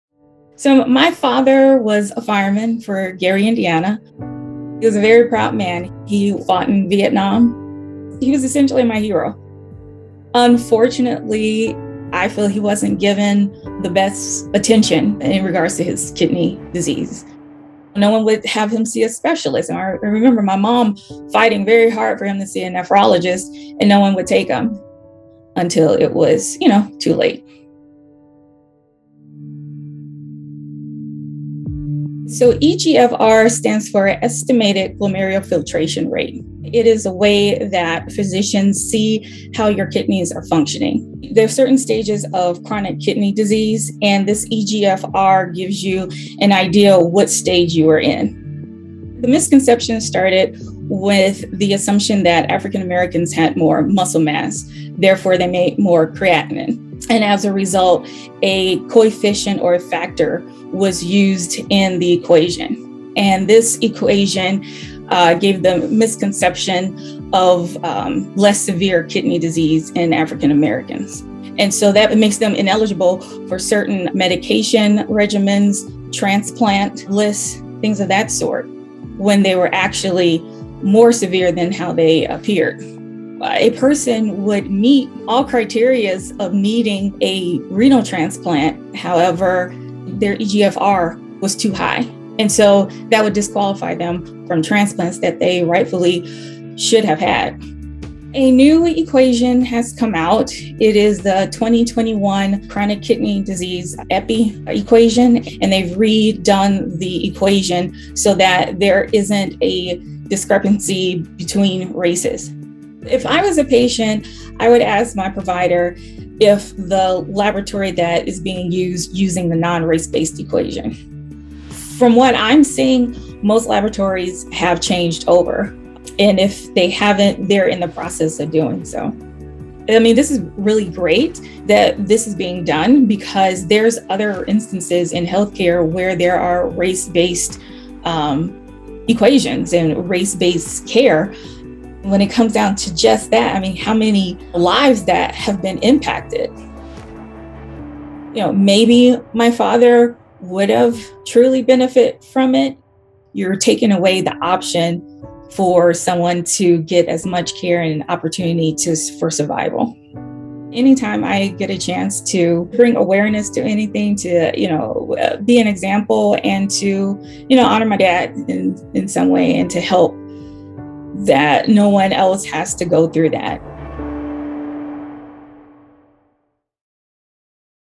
Recalculating the Role of Race in Kidney Disease Diagnosis
La’Tonzia Adams, MD, MS, FCAP, witnessed how an artificially heightened estimated glomerular filtration rate (eGFR) prevented her father from receiving adequate kidney disease treatment. Now, Dr. Adams is spreading the word about the elimination of race-based adjustment factors from eGFR equations, as well as the need to evaluate race’s impact on patient care in other areas of medicine.